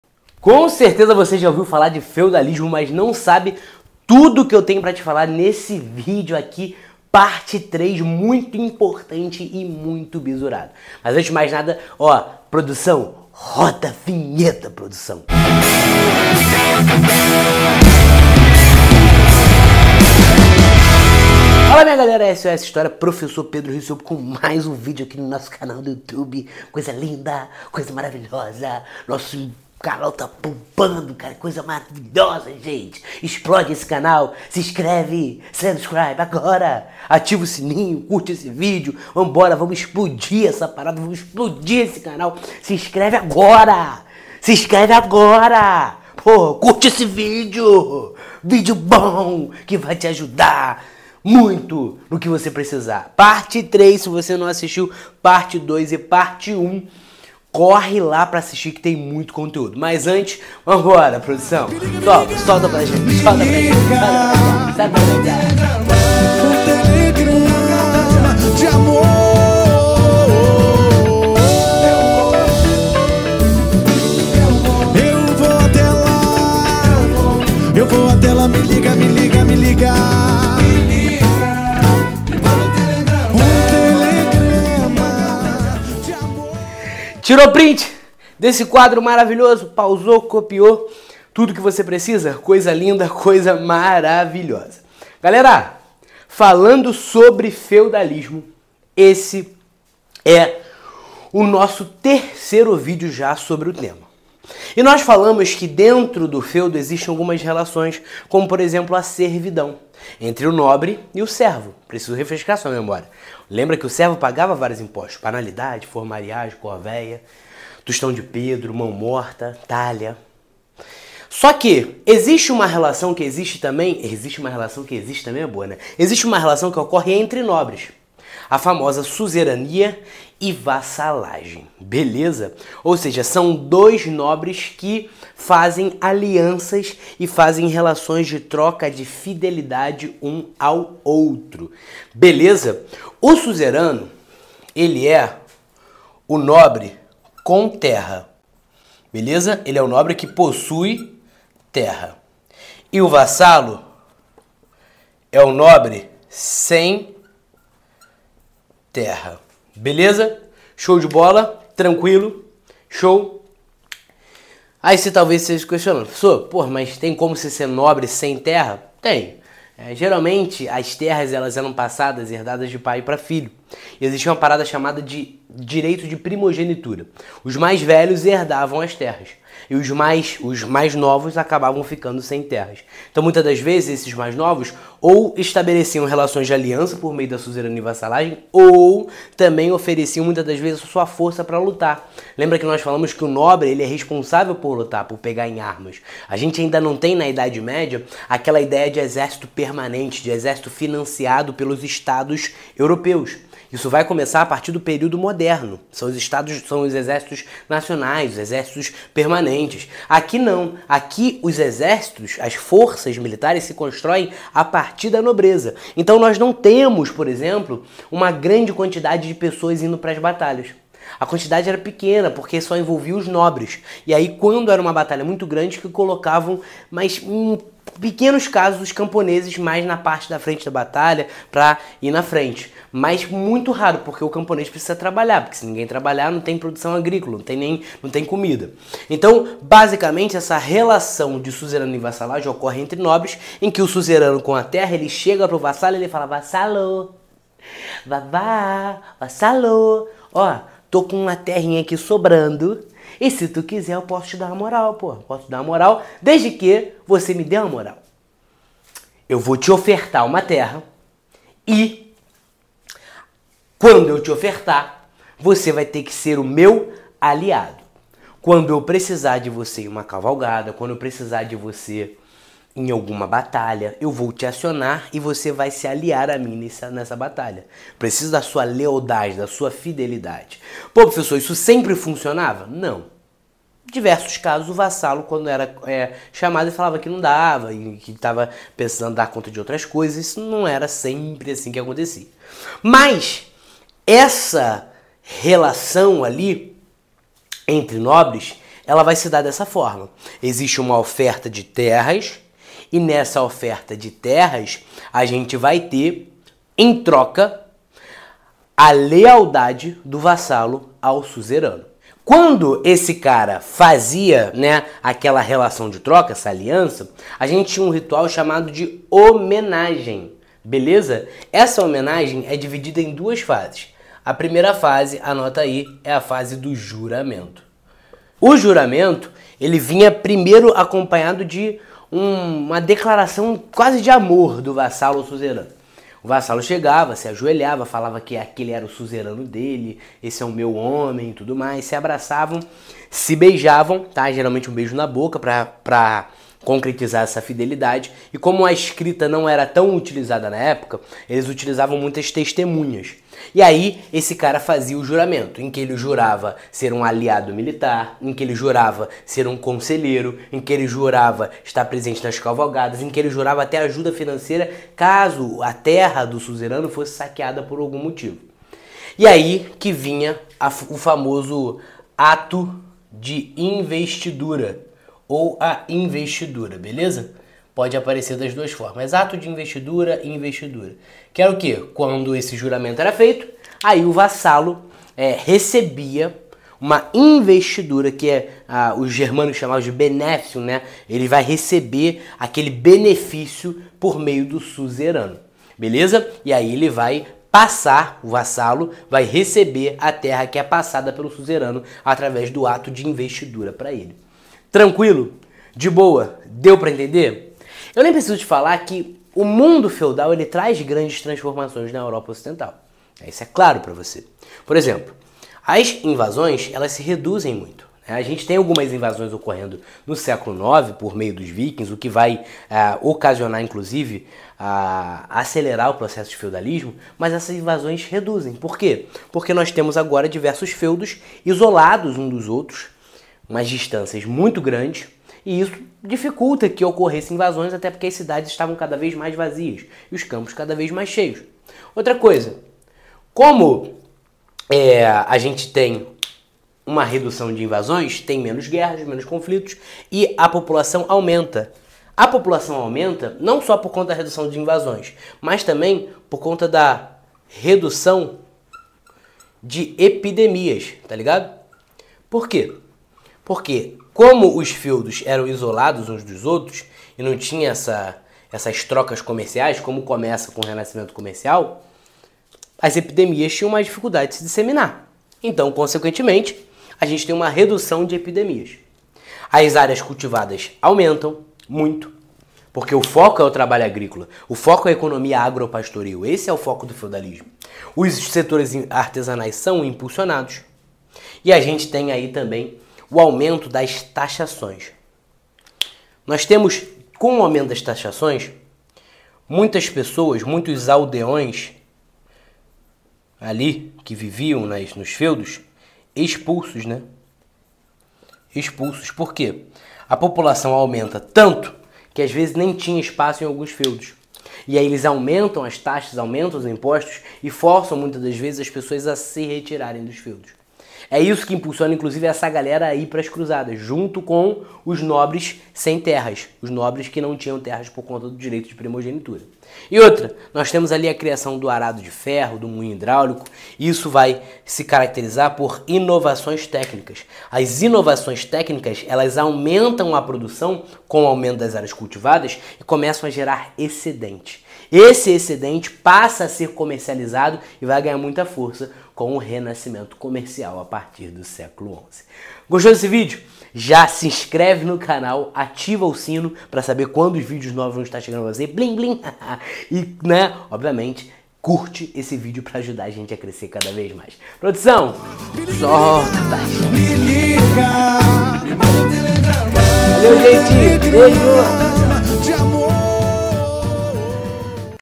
ENTENDA O FEUDALISMO (PARTE 3) - SOS História {Prof.Pedro Riccioppo}
Falaaa minha galera! Tudo bem com vocês? Hoje o vídeo é a terceira parte de Feudalismo, você sabe o que é?